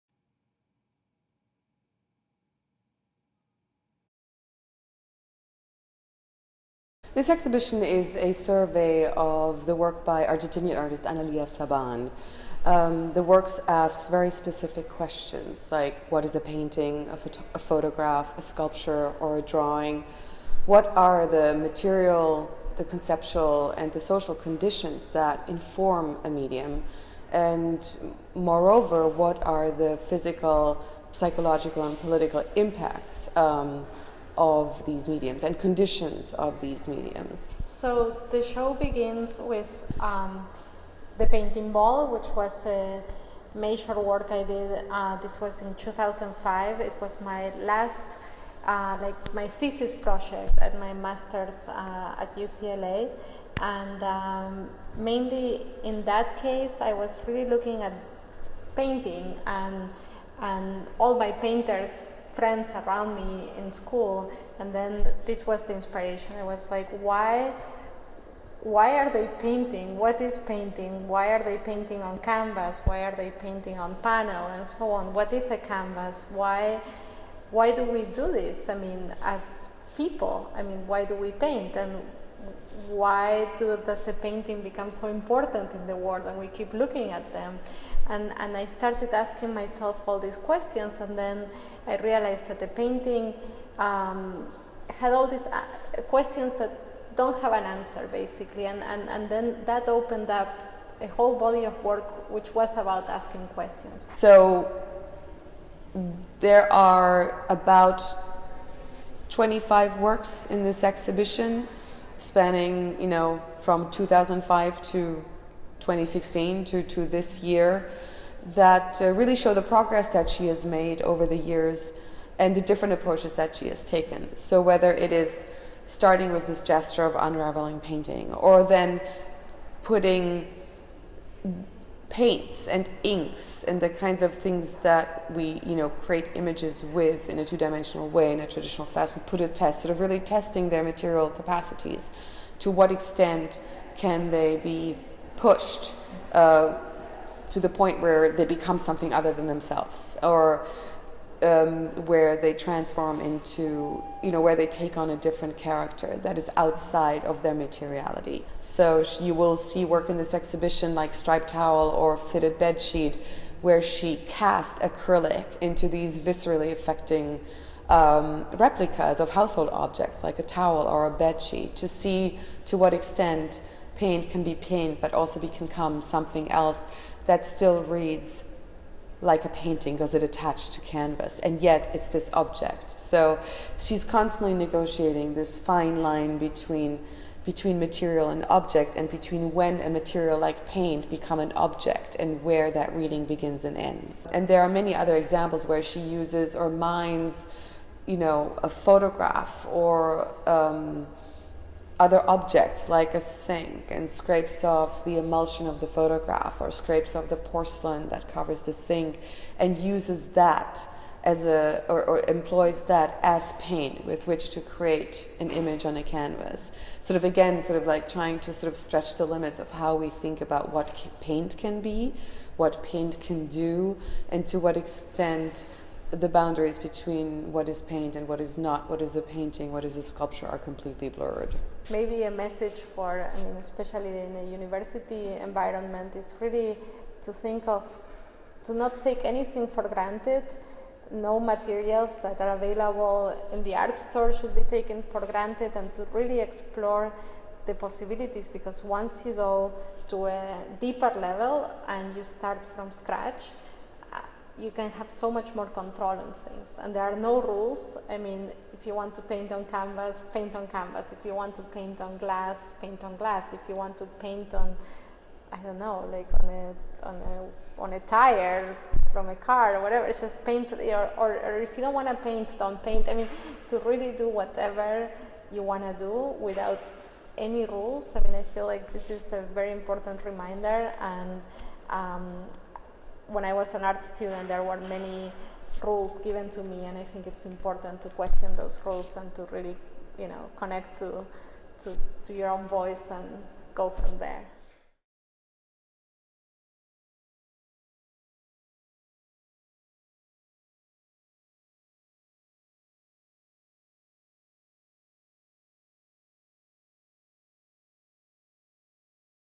Analia Saban at Blaffer Art Museum, 2016-2017
Introductory video on Analia Saban, exhibited at Blaffer Art Museum (September 27, 2016 - March 18, 2017) featuring interviews with the artist as well as the exhibition's curator, Claudia Schmuckli, then Blaffer's Director and Chief Curator.